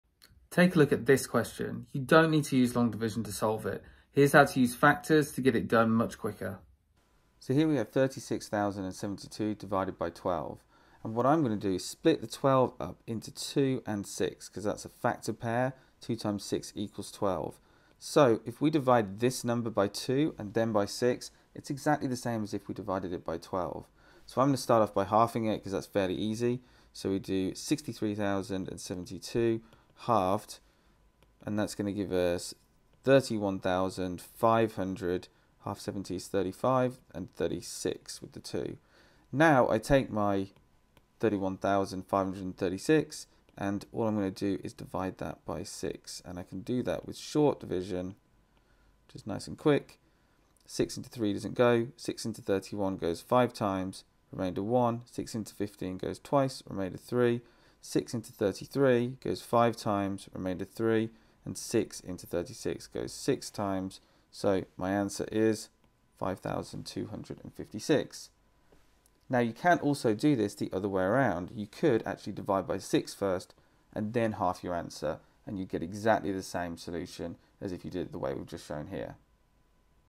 📚Factors Division trick📚
You don’t need to use long division if you can find a factor pair instead!